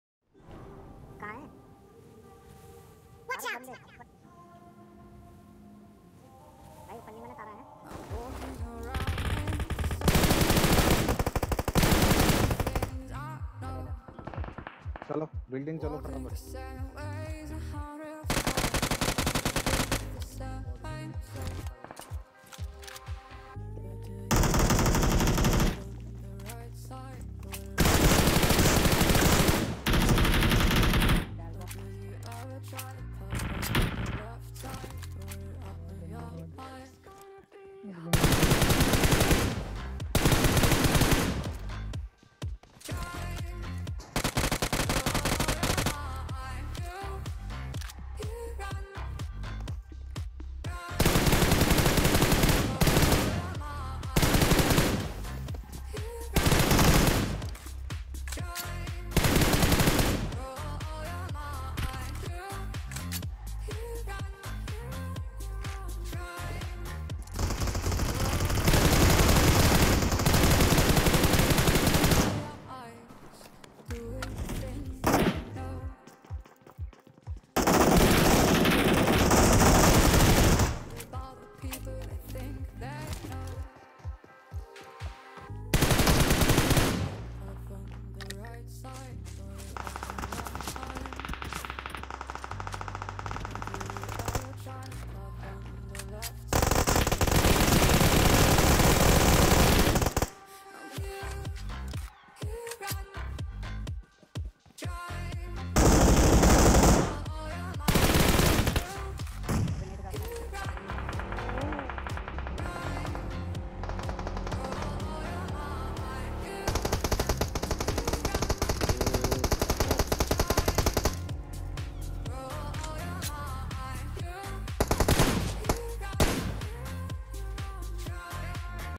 New rush gameplay chicken dinner with 6 kills
Chicken dinner 🍽️ best fight military base